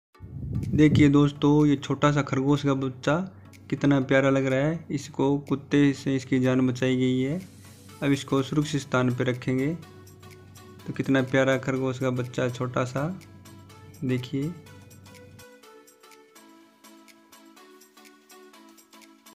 खेत मे खरगोश के बच्चे की रक्षा की//Protected The Baby Rabbit in The Field.
VISITING RABBIT RANCH -The
elevated bamboo breeding se.

videos!